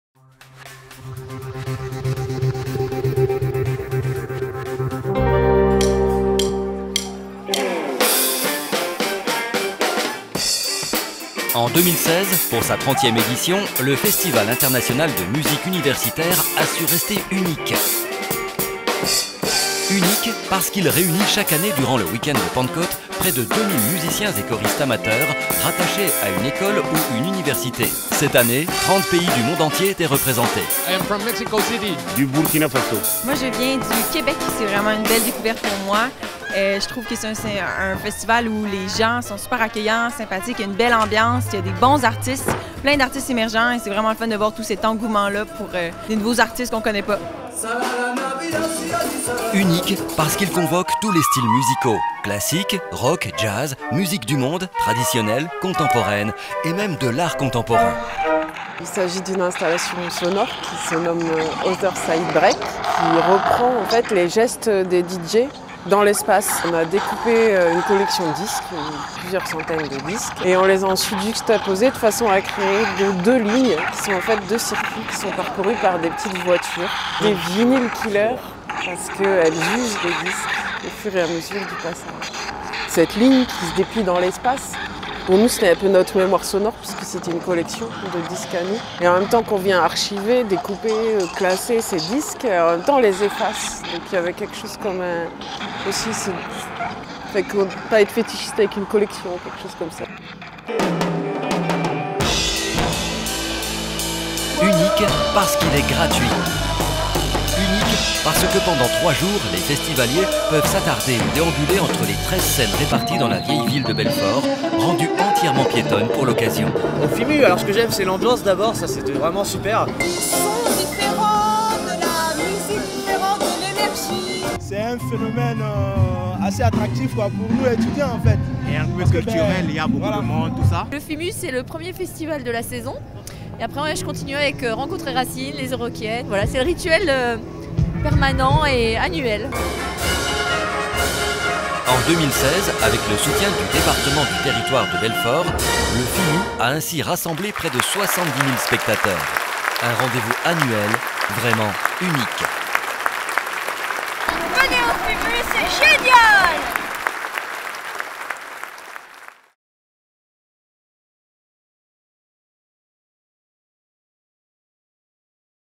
FIMU 2016
Chaque week-end de Pentecôte, la ville de Belfort se transforme en une gigantesque scène musicale internationale à ciel ouvert avec le Festival international de musique universitaire (FIMU).